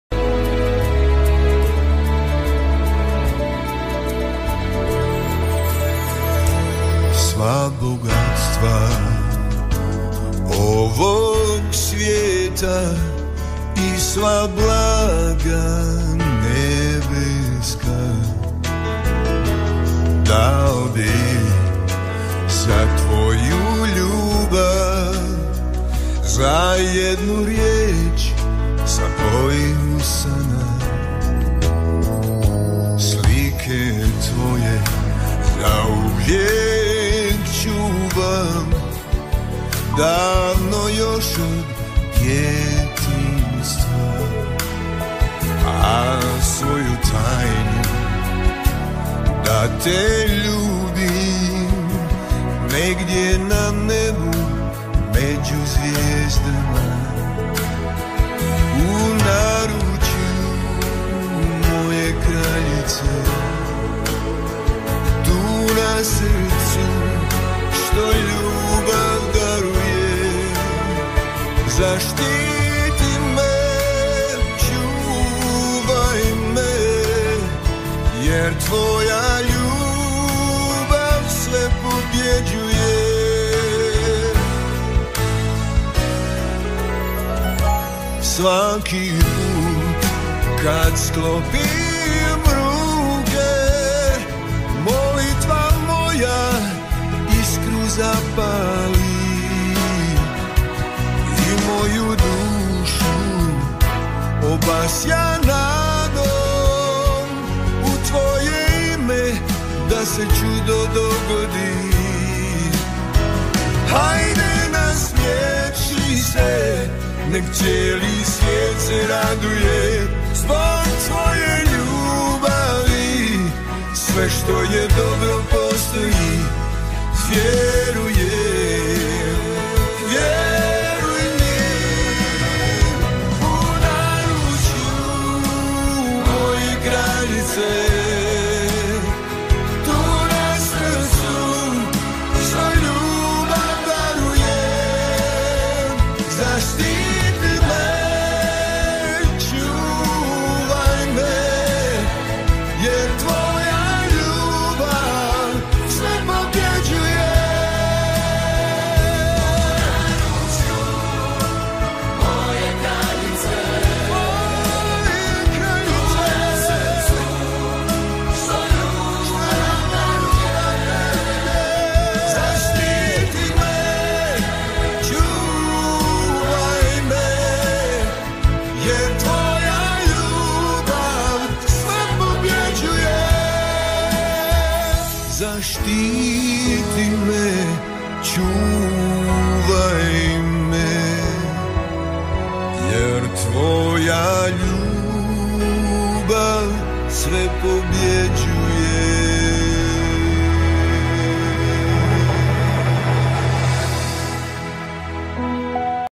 Radio Mir Medjugorje - Best Station On This Planet!
Gelobt sei Jesus Christus,

hier ist wieder aus dem Heiligsten Ort dieser Welt ein Richtig tolles Lied von der Radiostation Mir Medjugorje!
Die schönen Photo's sind wie üblich von Rudolf Baier, Friedberg, Deutschland, der sich gerade dort befindet!
Vielen Dank lieber Rudi dass Du mich immer mit Richtig guten Photo's versorgst, wir sind wirklich ein unschlagbares Team!

In Medjugorje ist wirklich Gott am Werk!